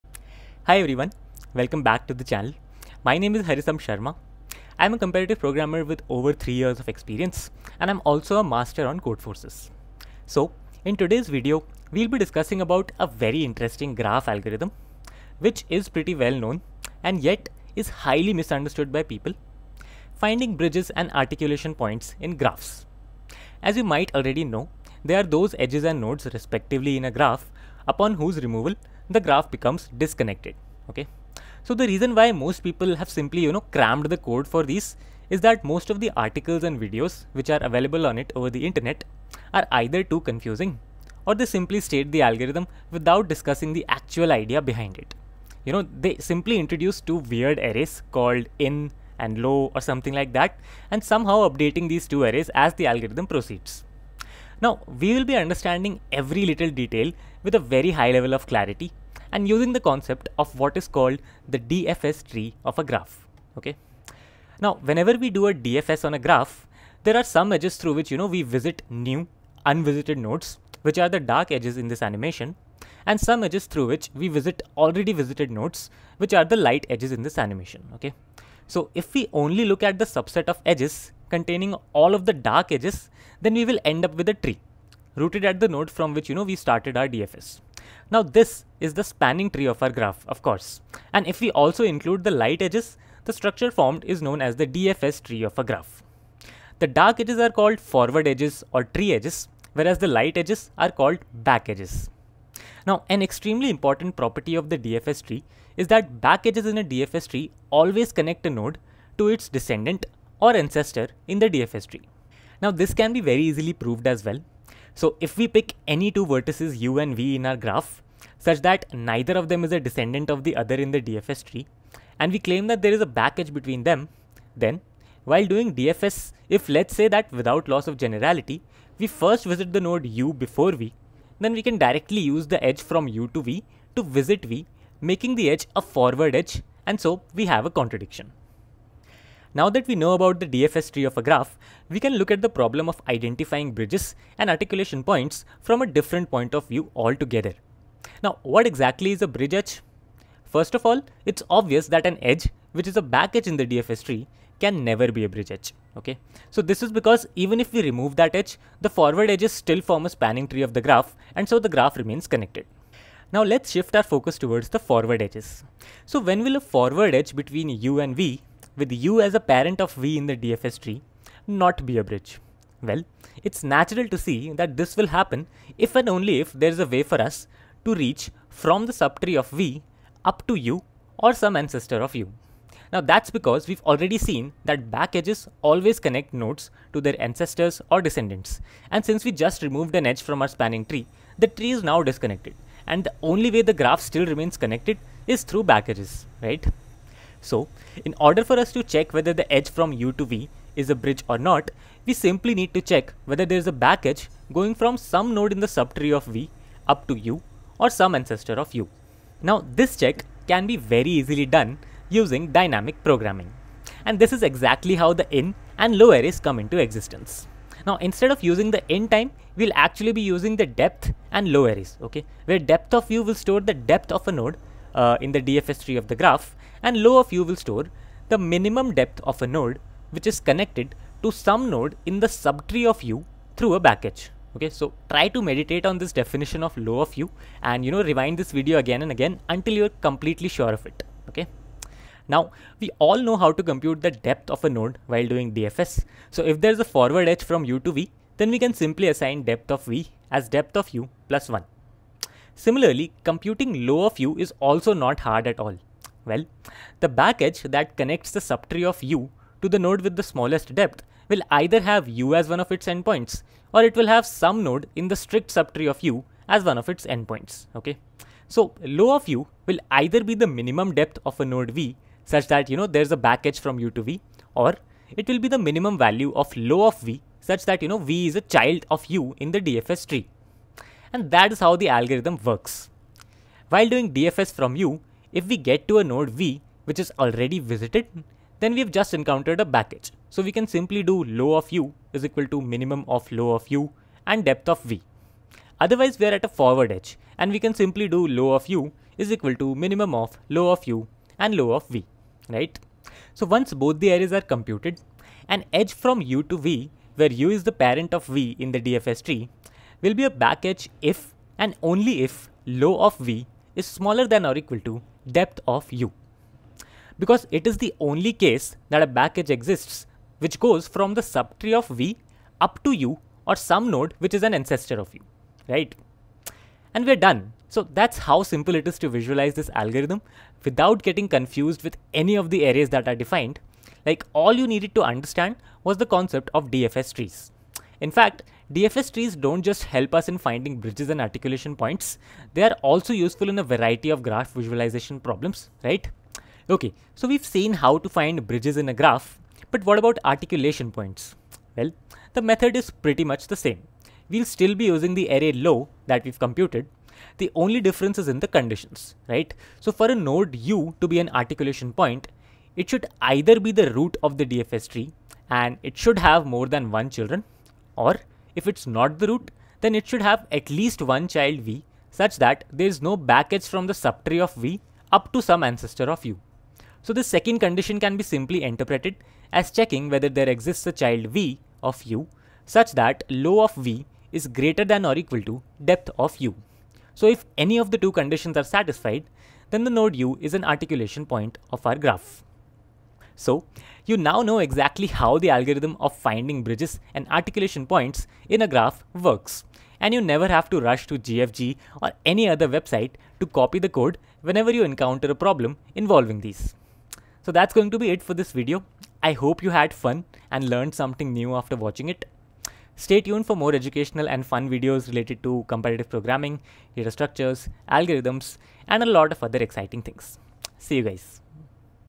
Finding Bridges and Articulation points in Graphs || How it ACTUALLY Works!
You can find the code for printing all Bridges and Articulation Points

In this comprehensive tutorial, I have done a dive deep to demystify the process of finding Bridges and Articulation points in a Graph. If you've ever found existing explanations confusing or overly abstract, you're in the right place!

Timestamps :
0:00 - Introduction
1:00 - What is a DFS Tree?
2:20 - Using DFS Trees to find Bridges
5:50 - Using DFS Trees to find Articulation Points